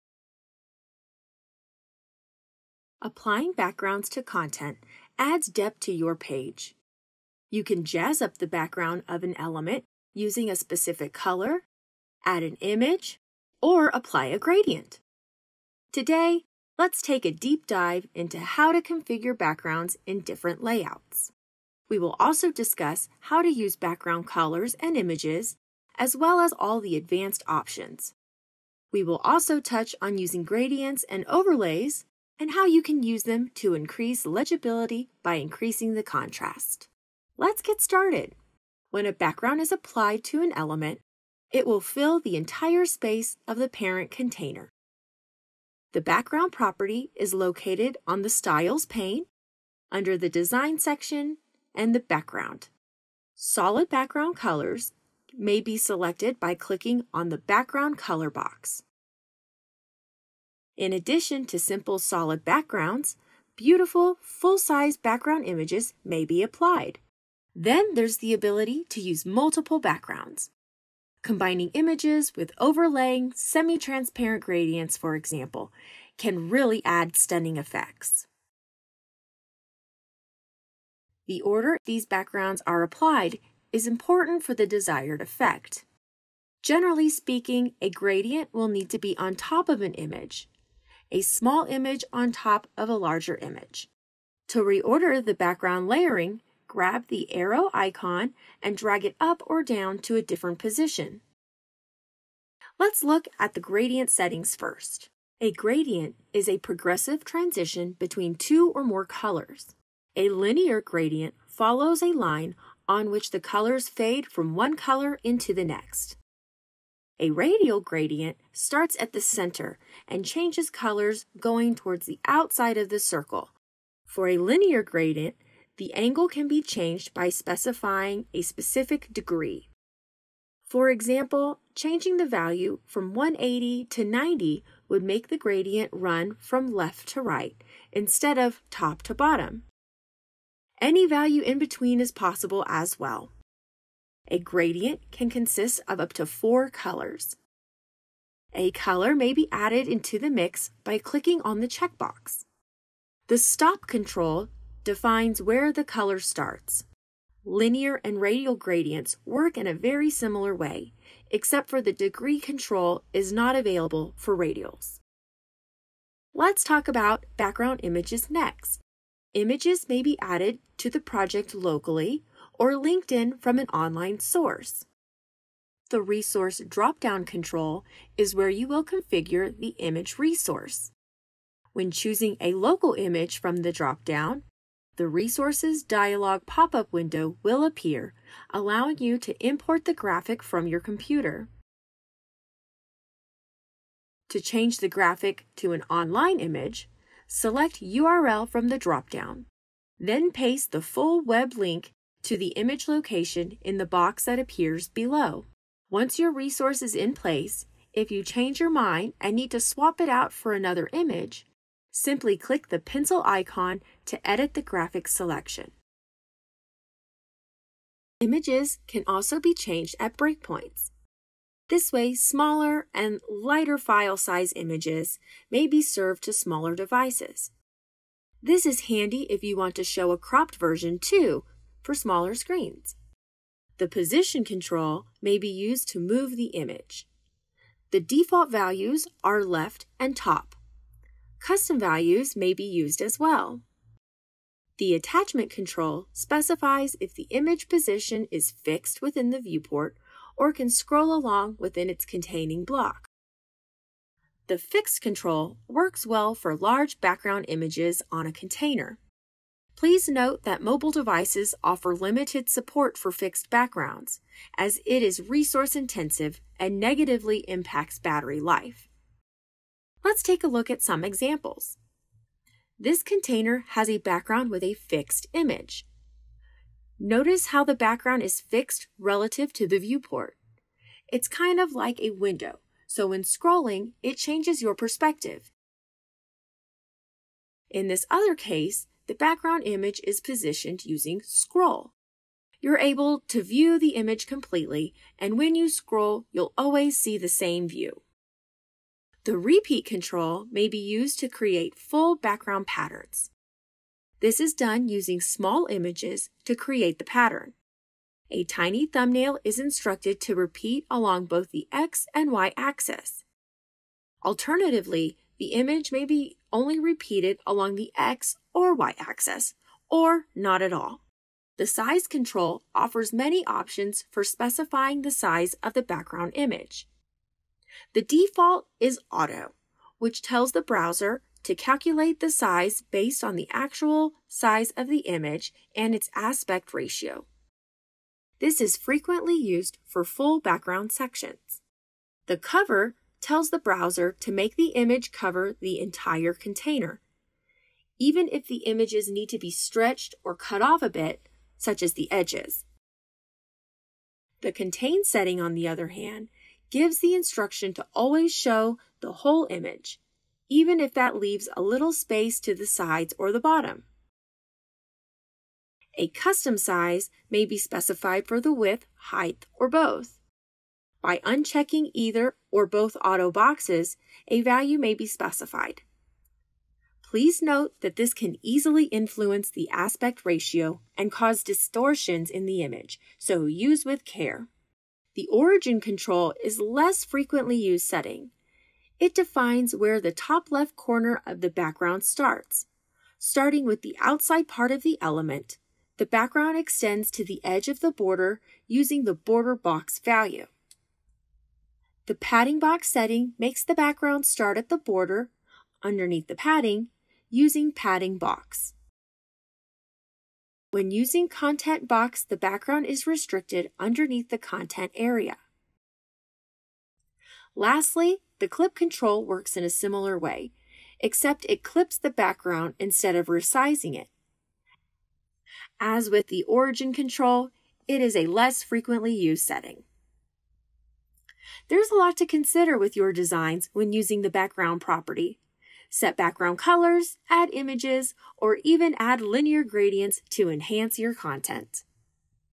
Backgrounds
Jazz up the Background of an element in Site Designer using a specific color, add an image, or apply a gradient.